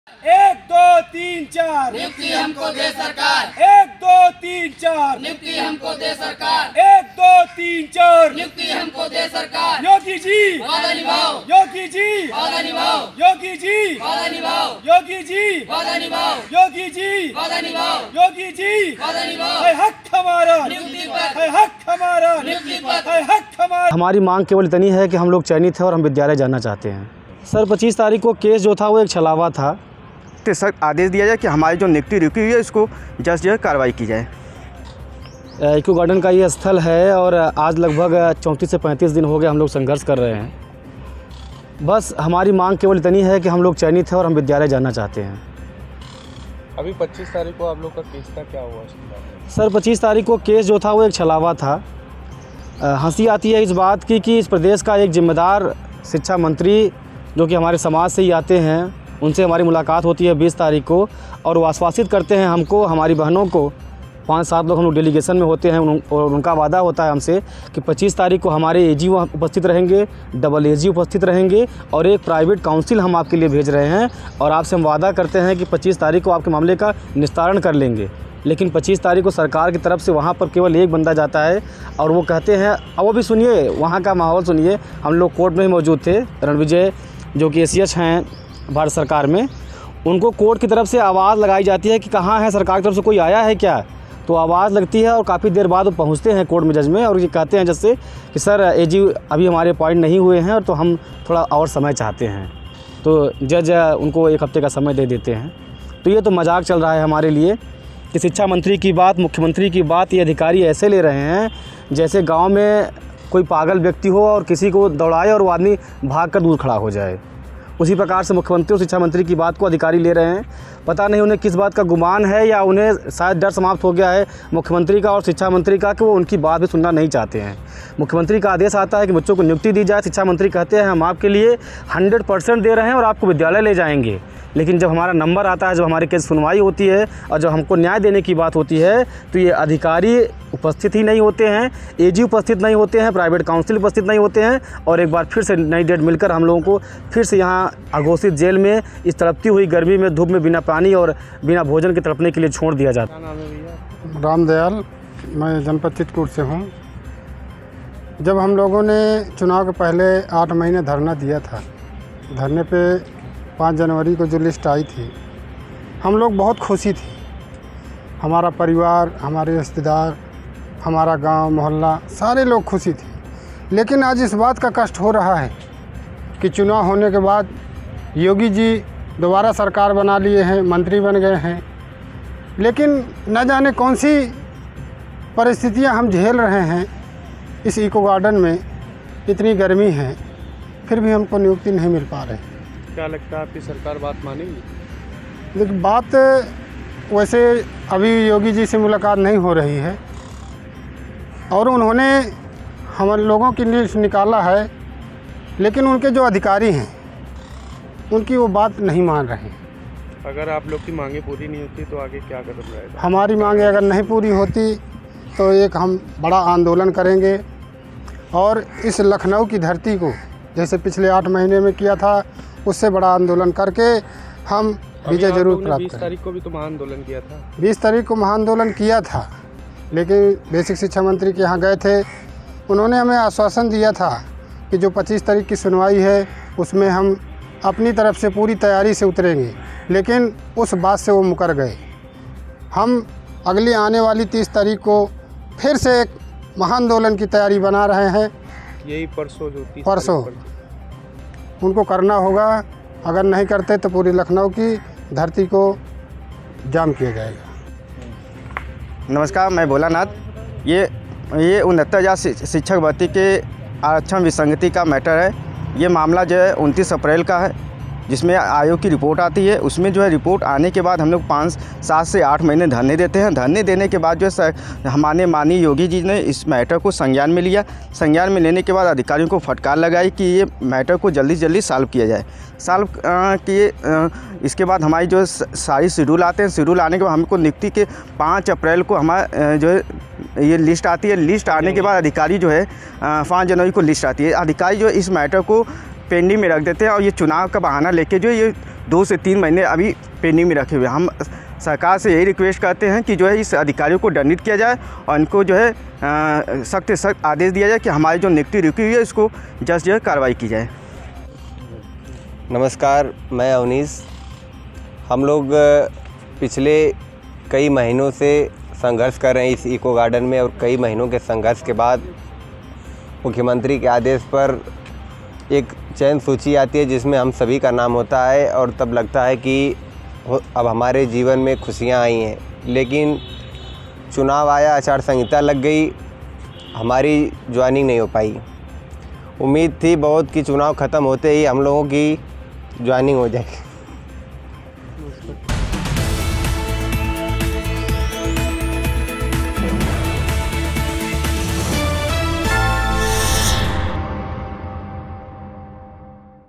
6800 शिक्षक भर्ती की नहीं हुई कोई सुनवाई, 30 अप्रैल को दुबारा महाआंदोलन
6800 शिक्षक भर्ती की नहीं हुई कोई सुनवाई, 30 अप्रैल को दुबारा महाआंदोलन |